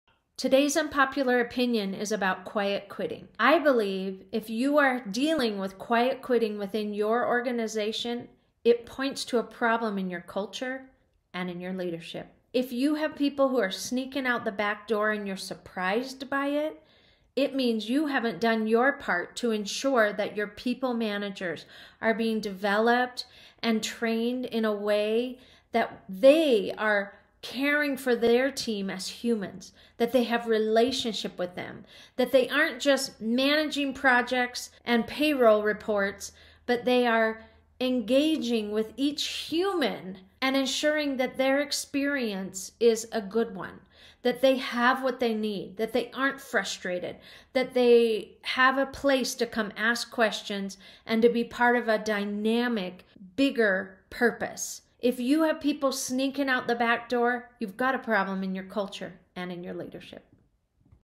Dealing with Quiet Quitting in your Organization?
Dealing with Quiet Quitting in your organization?  It might be a problem with your culture and leadership.

What are you doing to ensure the Human Experience within your organization is one that offers your team members support, training, a safe place to share ideas or issues, that they are being celebrated, heard and seen? If people are quitting and you are surprised, check your culture and your leadership.

Good leaders will have relationship with their team and be able to recognize the signals. Are you empowering and developing your people managers to be human centric?